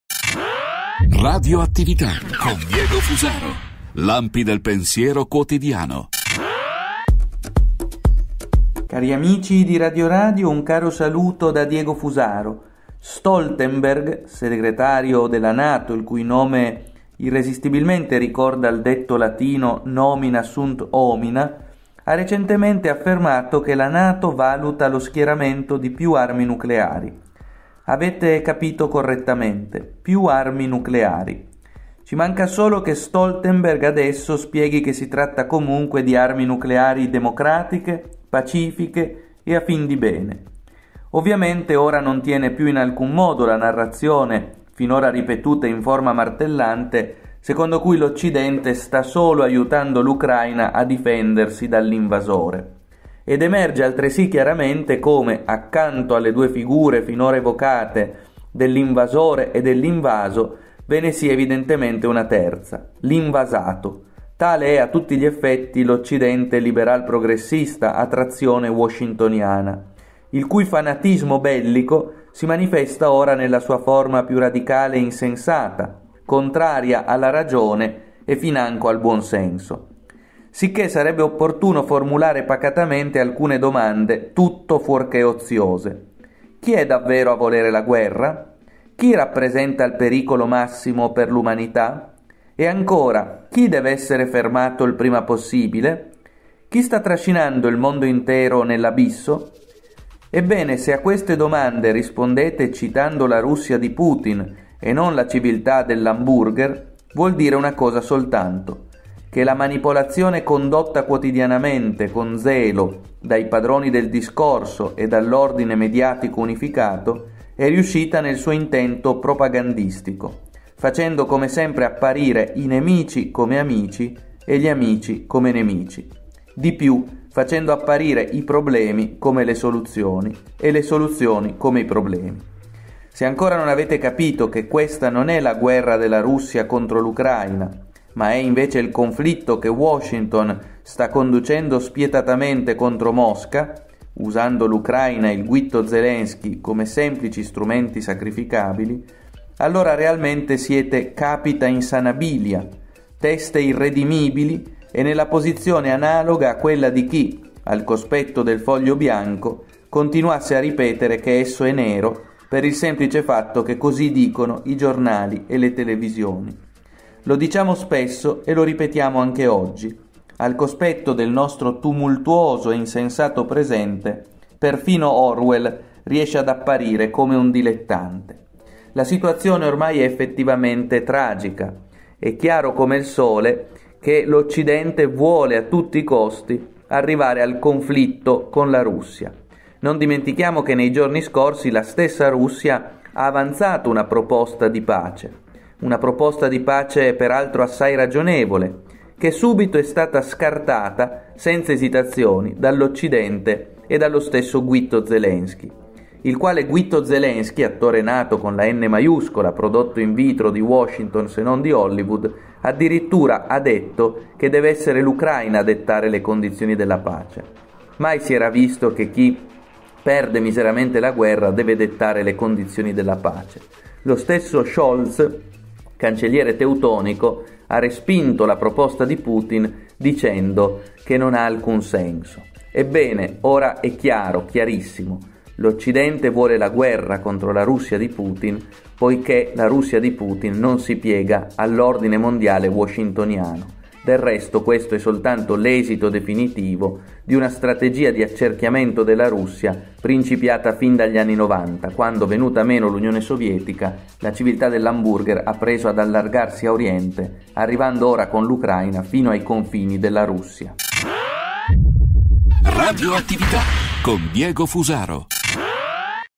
Nonostante la proposta di pace della Russia, la Nato sta valutando lo schieramento del nucleare
Stoltenberg , segretario della Nato il cui nome irresistibilmente ricorda il detto latino Nomina sunt homina, ha recentemente affermato che la Nato valuta lo schieramento di più armi nucleari. Avete capito correttamente: più armi nucleari. Ci manca solo che Stoltenberg spieghi che si tratta di armi nucleari democratiche, pacifiche e a fin di bene. Ovviamente ora non tiene più la narrazione, ripetuta in forma martellante, secondo cui l'Occidente sta solo aiutando l'Ucraina a difendersi dall'invasore.

Ed emerge altresì chiaramente come, accanto alle due figure finora evocate dell'invasore e dell'invaso, ve n'è una terza, quella dell'invasato. Tale è a tutti gli effetti l'occidente liberal-progressista a trazione washingtoniana, il cui fanatismo bellico si manifesta ora nella sua forma più radicale e insensata, contraria alla ragione e al buonsenso. Sicché sarebbe opportuno formulare pacatamente alcune domande, tutto fuorché oziose.